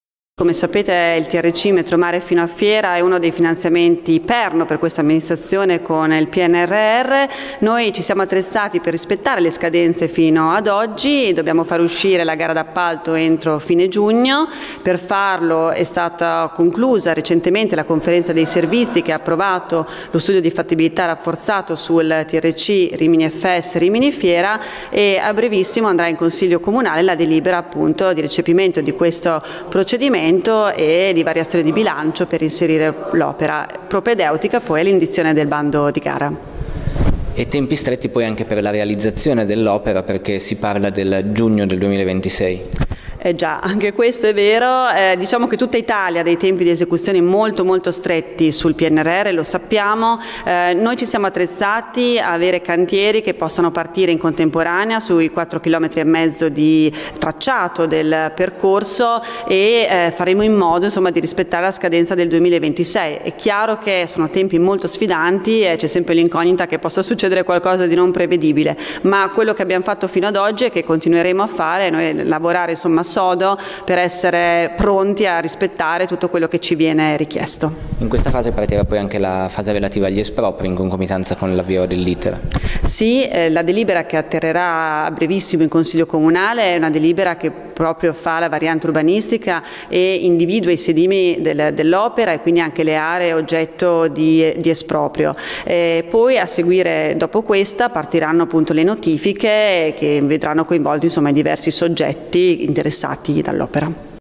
Metromare, conclusa conferenza servizi. Bando entro giugno, contratto entro l'anno
Si corre per rispettare i tempi del Pnrr. L'amministrazione riminese ribadisce i tempi della seconda tratta del Metromare che collegherà la stazione alla Fiera. Contratto entro fine anno, cantiere ultimato per il 30 giugno 2026 (Icaro TG del 07/06/2023)